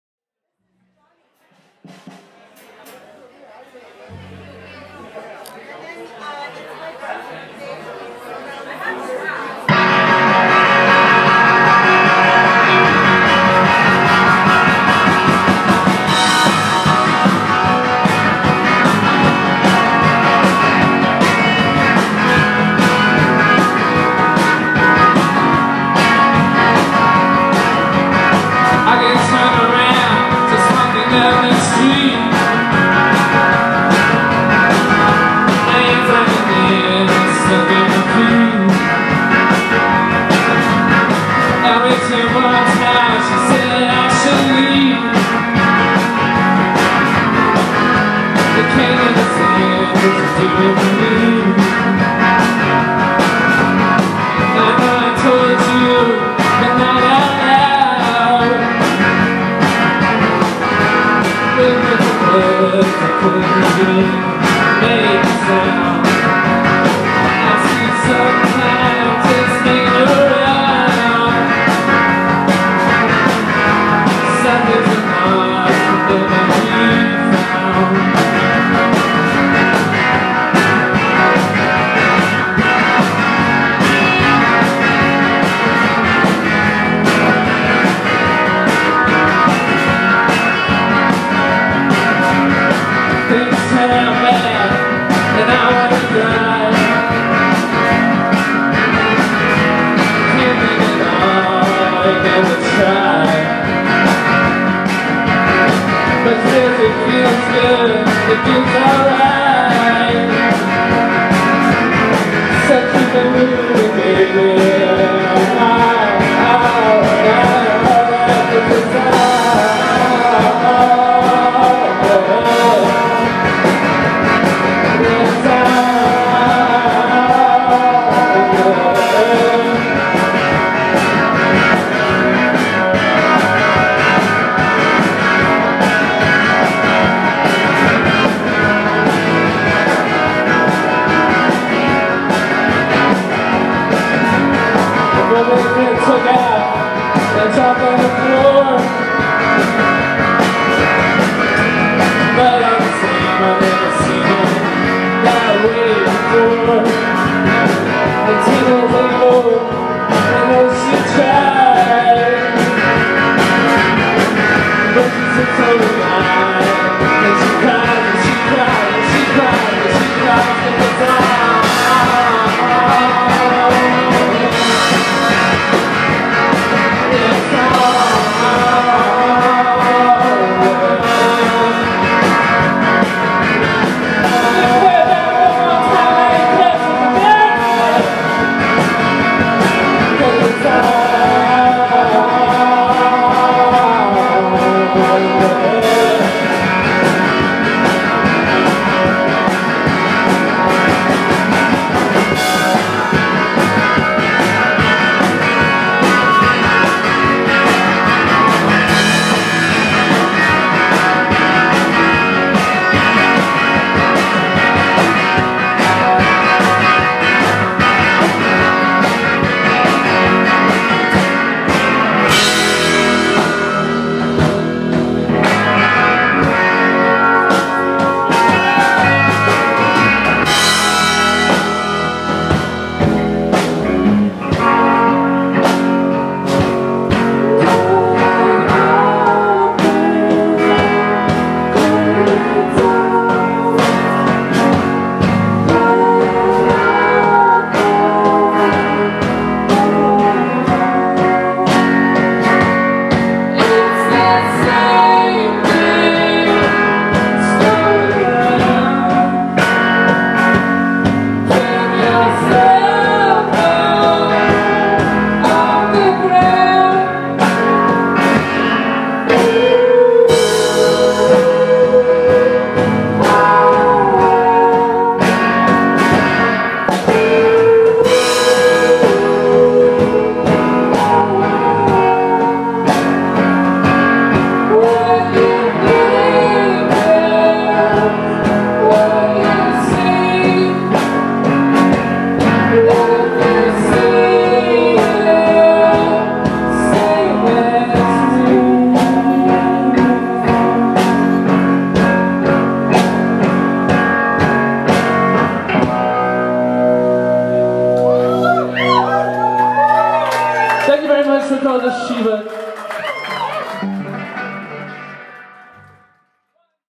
The Shivas Live at The Know
Portland, OR
11, 17, 2016

Jared Molyneux
Eric Shanafelt
Kristin Leonard